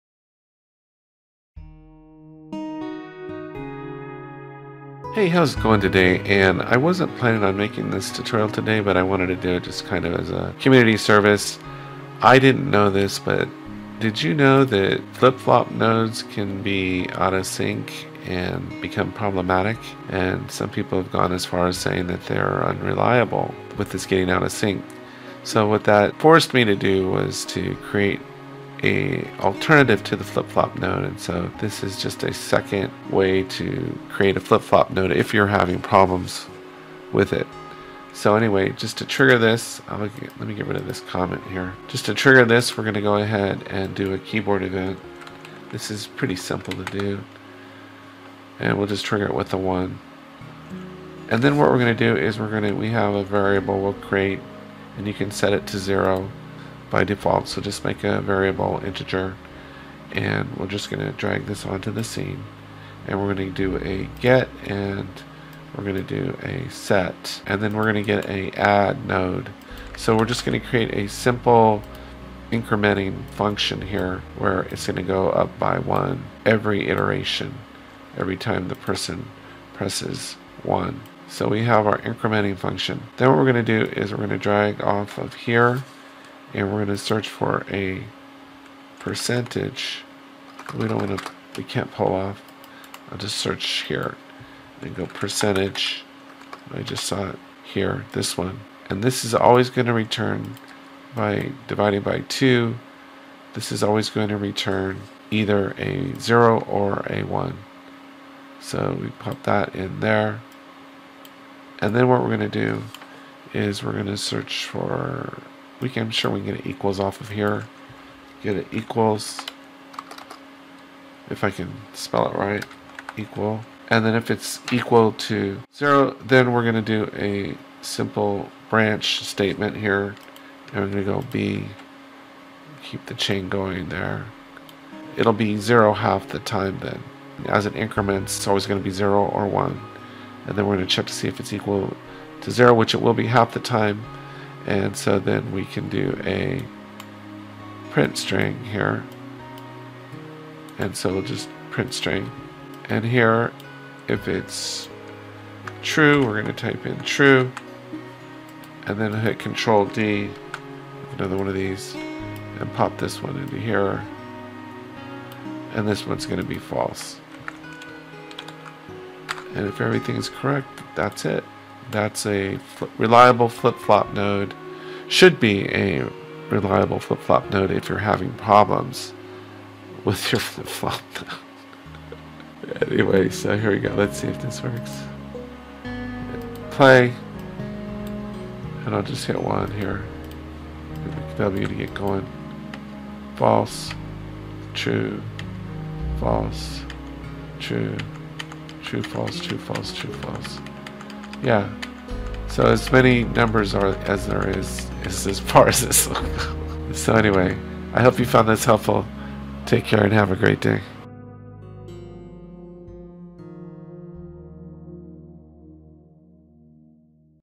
Easy Alternative to Flip Flop Node in Unreal Engine
Easy way to make an alternative flip flop node if you are having problems with the built-in one.